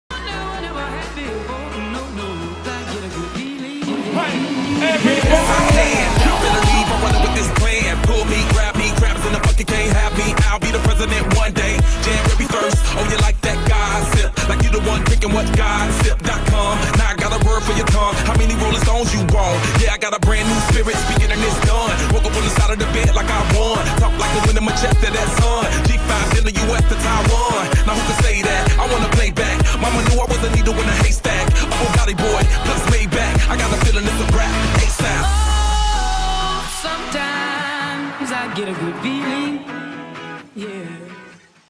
Edmonton Eskimos - Flo Rida Halftime Show - July 23, 2016
The Canadian Football League's Edmonton Eskimos brought in Billboard chart-topper and international artist FLO RIDA for a half time performance on July 23rd, 2016.